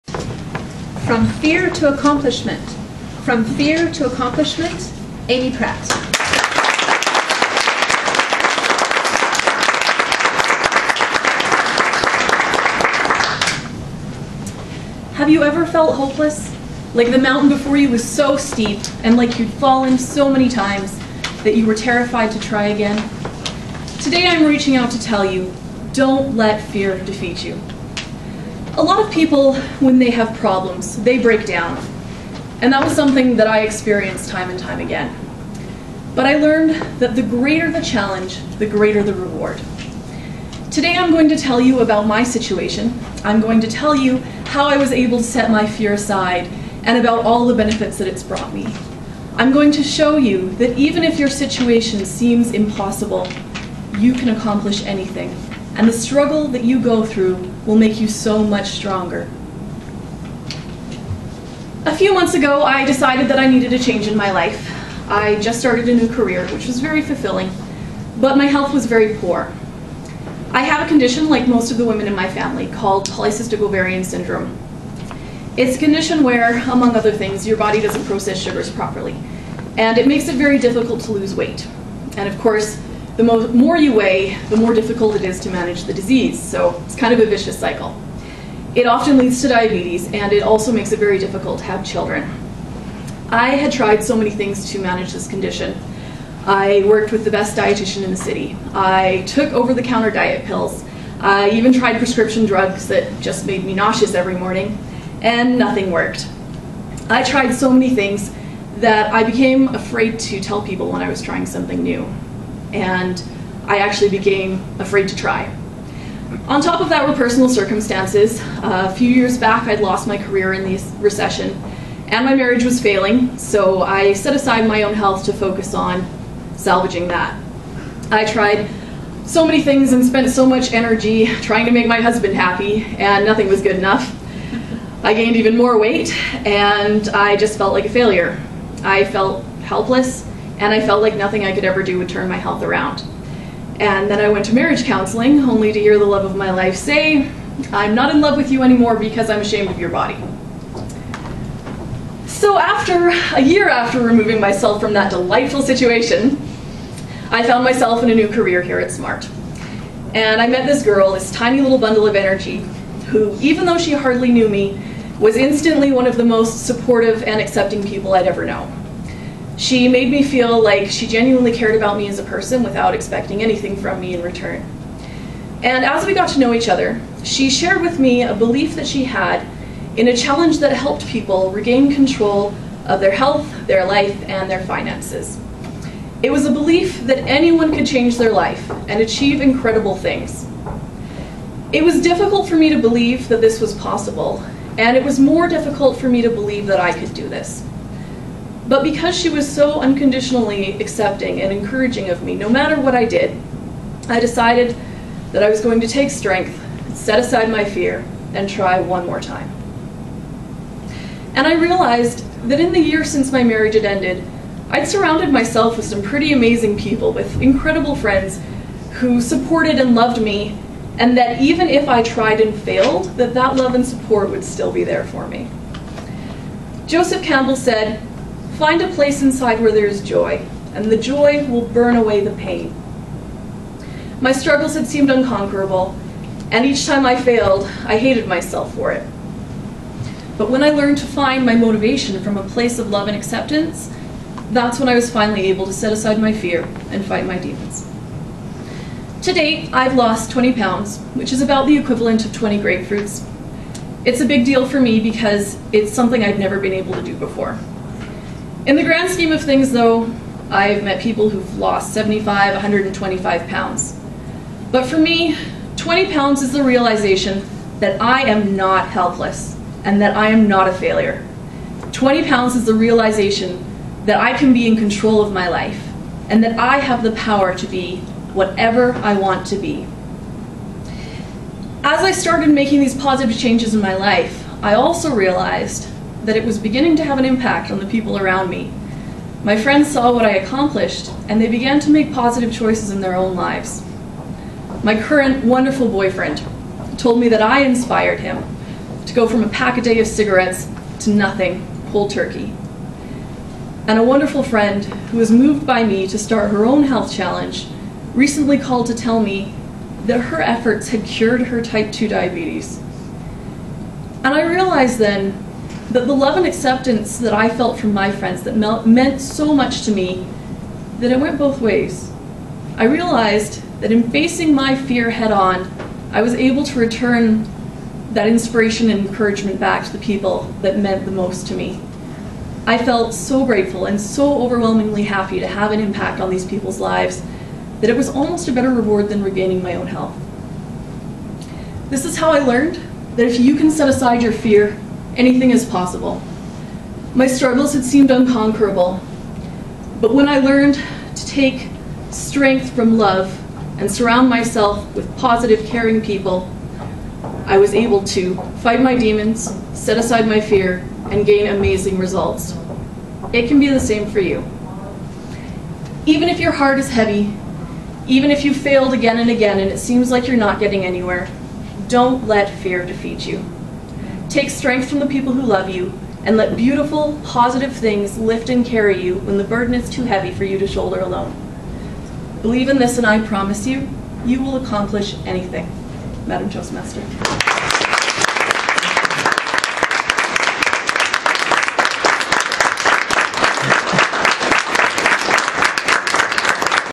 Toastmasters - Speech 6 - From Fear to Accomplishment
Speaker: Amy Pratt
Speech #6 - "Vocal Variety" - for the International Speech Competition (Inspirational)
Given to Toastmaster club "SMART Speakers" on March 8, 2012